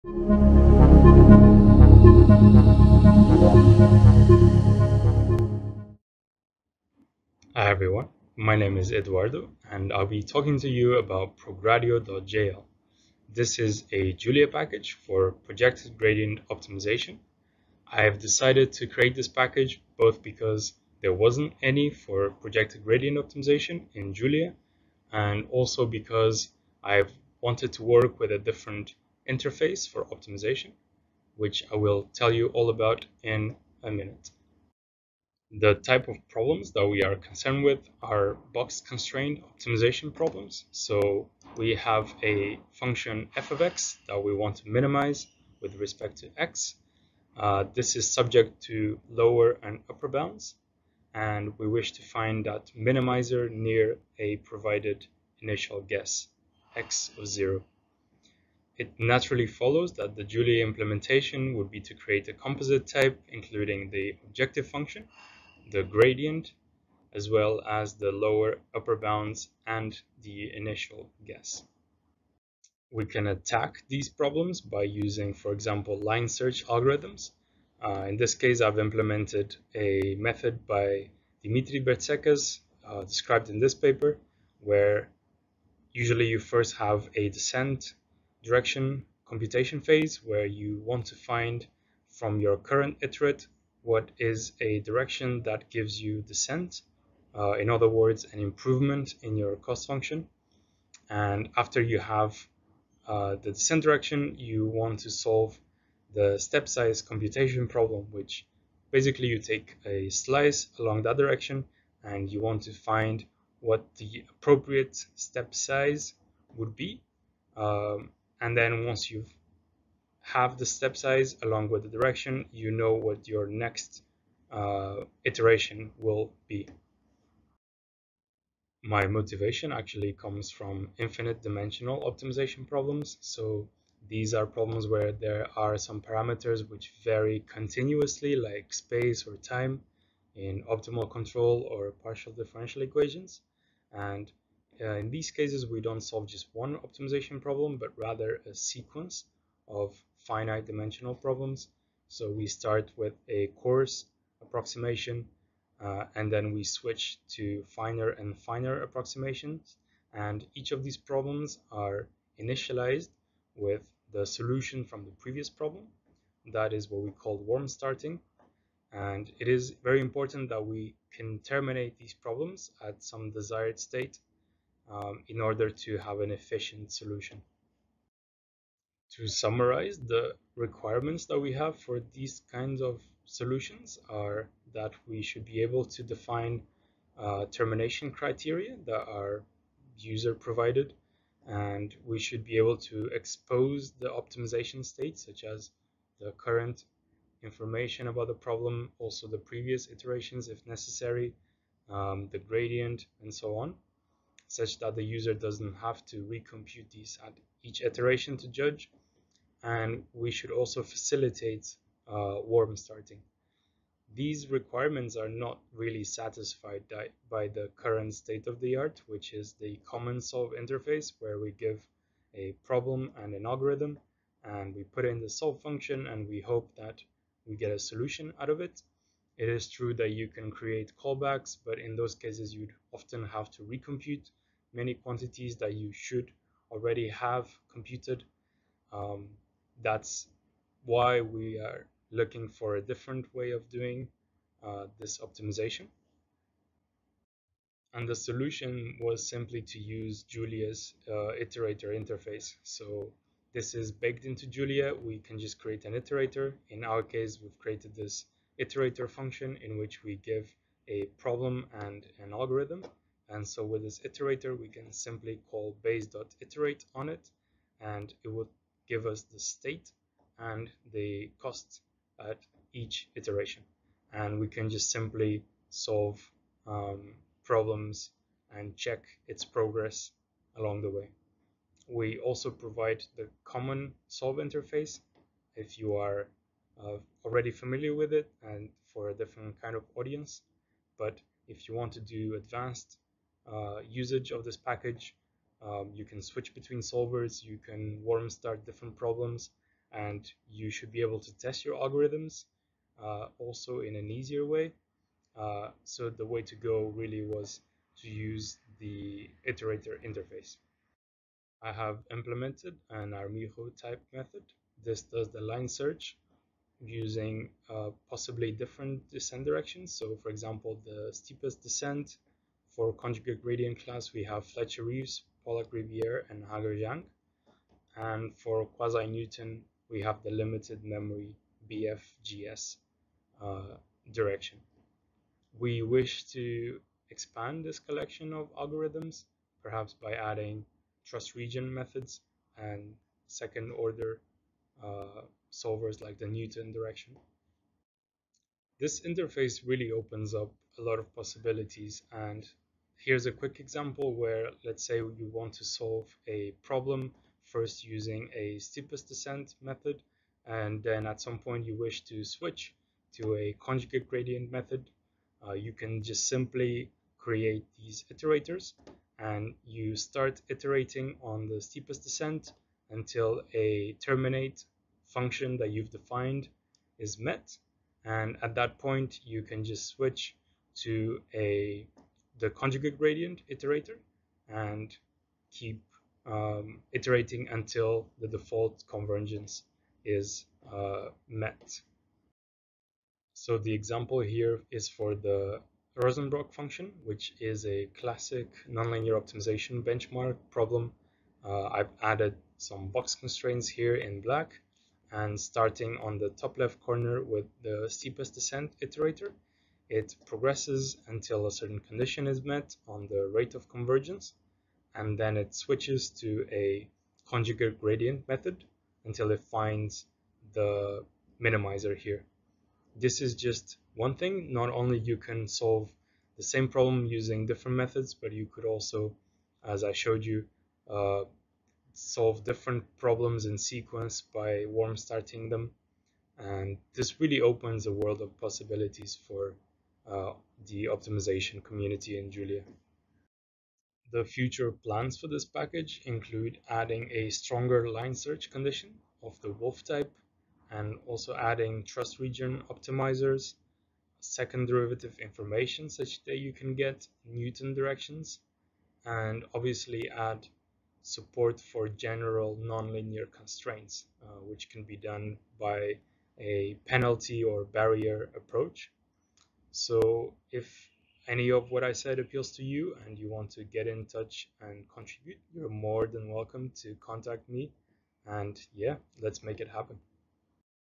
Progradio.jl: Projected Gradient Optimization | Eduardo M. G. Vila | JuliaCon 2022
Most (Mathematical) Optimization problems are subject to bounds on the decision variables. In general, a nonlinear cost function `f(x)` is to be minimized, with the vector `x` constrained by simple bounds. The *Projected Gradient* class of methods is tailored for this very optimization problem. Our package includes various Projected Gradient methods, fully implemented in Julia. We make use of Julia's Iterator interface, allowing for user-defined termination criteria.

00:00 Welcome!
00:10 Help us add time stamps or captions to this video! See the description for details.